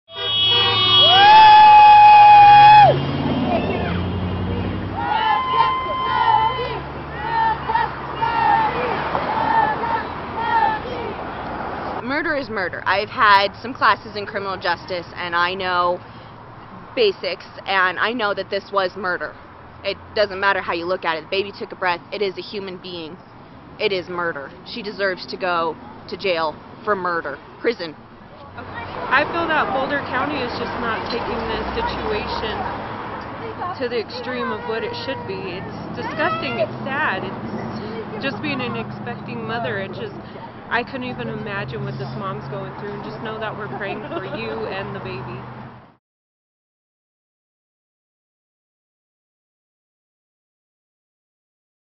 Longmont residents gathered on Thursday to protest for murder charges to be filed against the accuse
Longmont residents gathered on Thursday to protest for murder charges to be filed against the accused fetal abducion suspect Dynel Lane.

By: Jeremy Papasso - I am a staff photographer/videographer for the Boulder Daily Camera, newspaper in Boulder, Colorado.